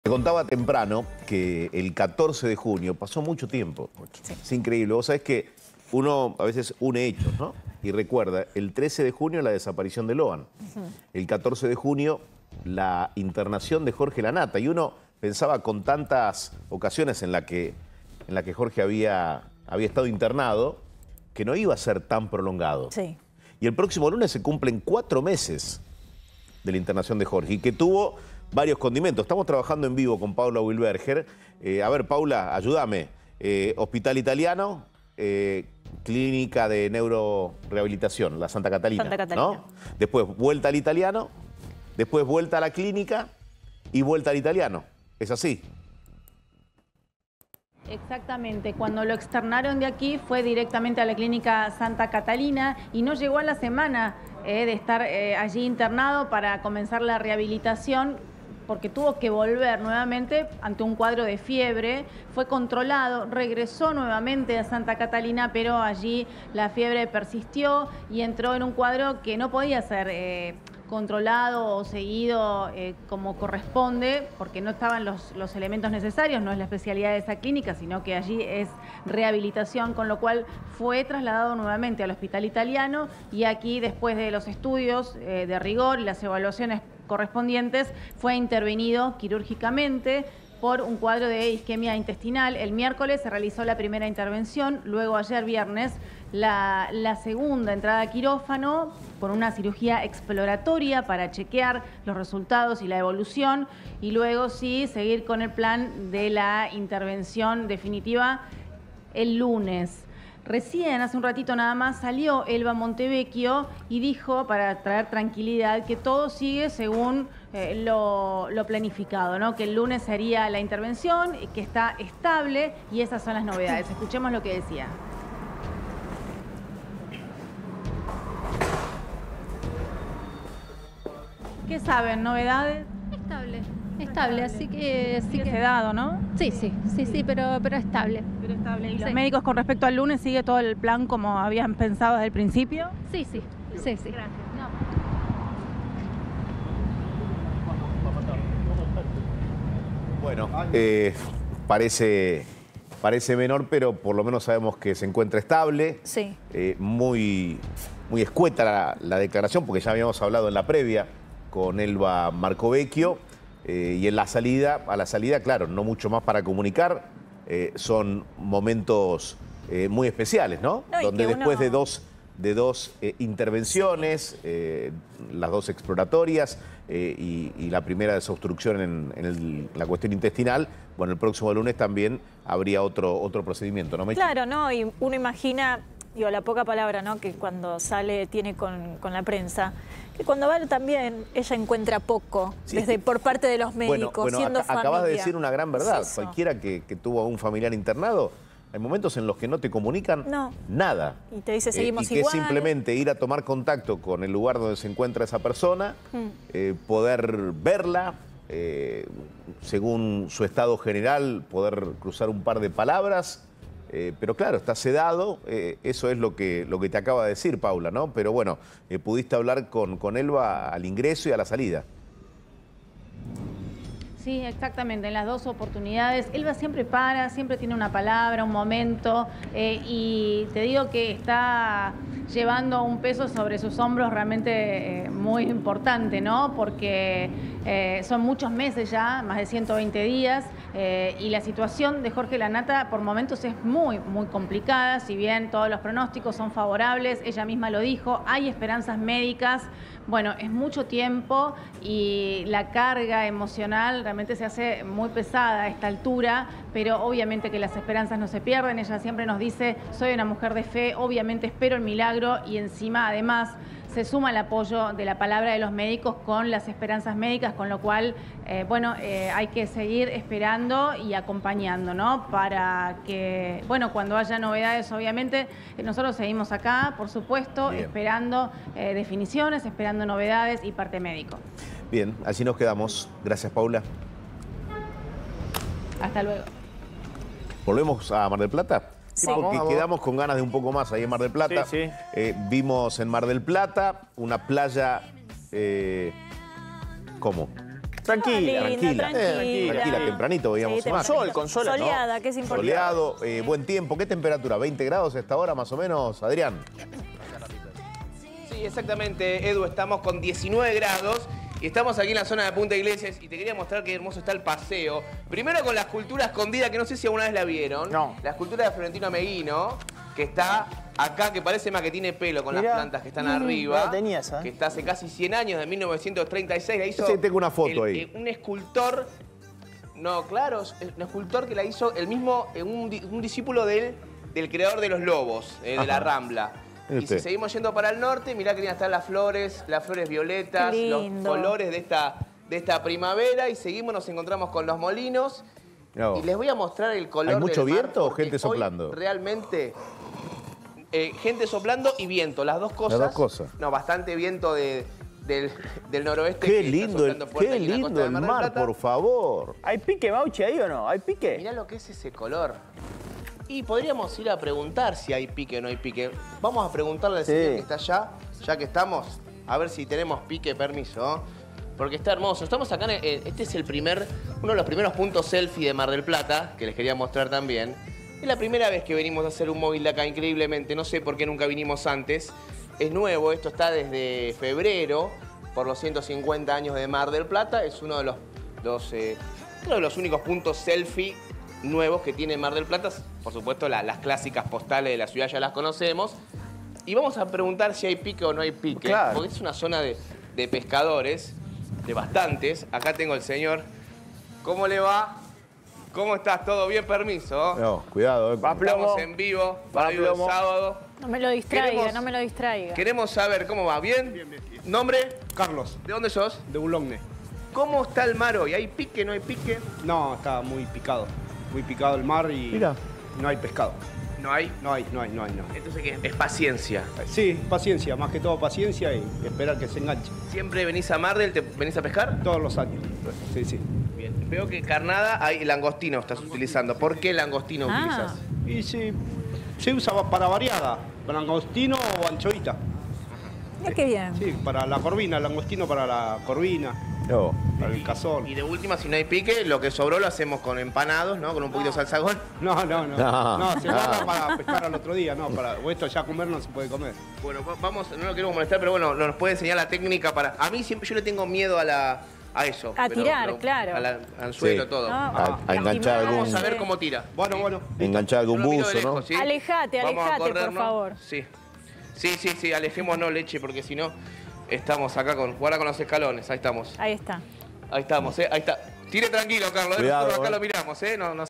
📣 ELBA MARCOVECCHIO HABLÓ EN EXCLUSIVA CON A24
Jorge Lanata continúa sedado tras casi cuatro meses de internación.

Fue operado y será intervenido nuevamente el lunes.